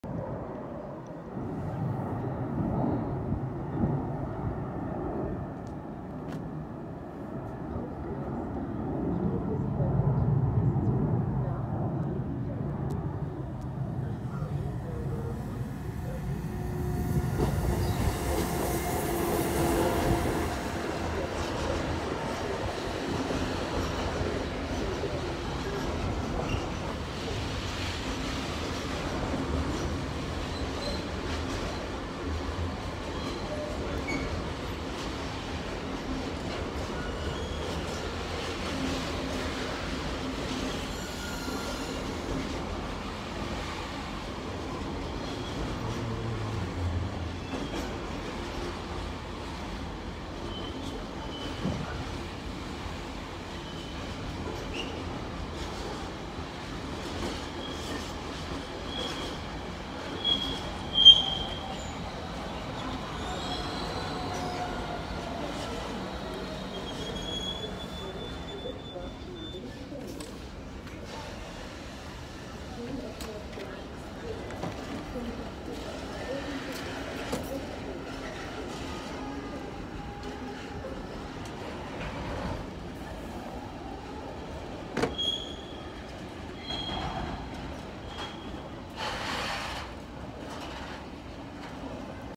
Güterzug der Firma "Metrans" und S-Bahn S2
Kurz nach dem EC filmte ich einen Güterzug der Firma Metrans mit einer Zuglok 186 und KV-Profil sowie eine einfahrende S-Bahn S2 von Marienfelde im Bf Blankenfelde.